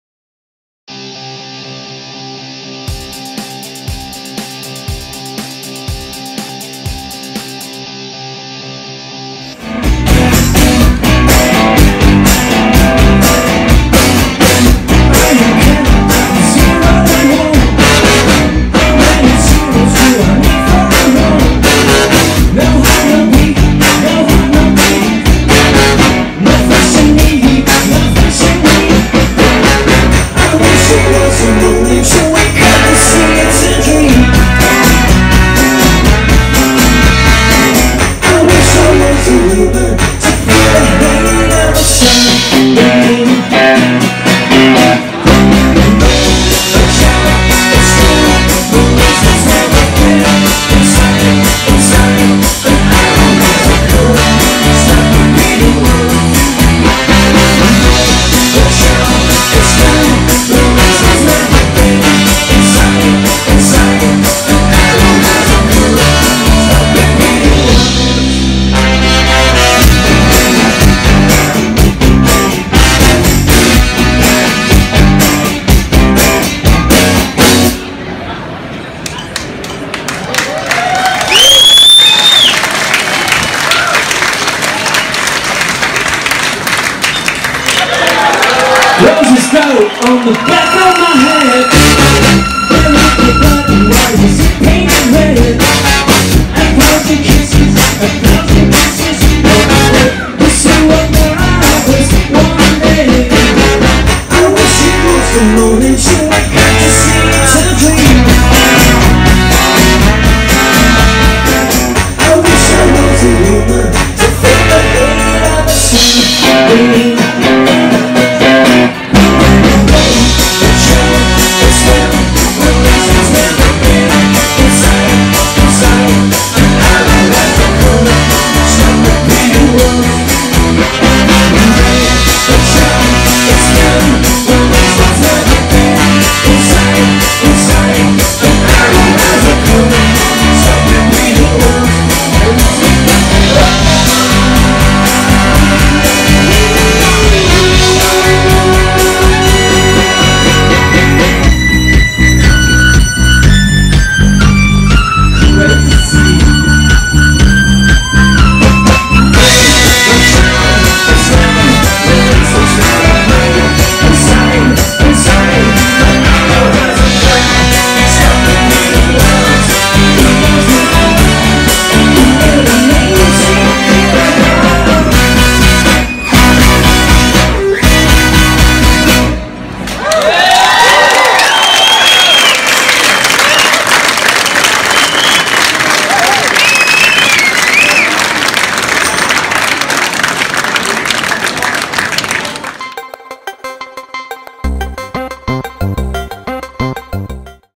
The Great Bertholinis - Live at Bardentreffen 2016
Als headliner standen am Freitag The Great Bertholinis auf der MUZ-Bühne am Lorenzer Platz beim Bardentreffen 2016. Mit Glitterschauer in der Mitte.